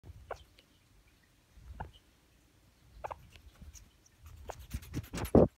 Wait for it….. 💕Skinny pig being adorable!💕
Sweet Poe does a dog impression…. Sniff sniff!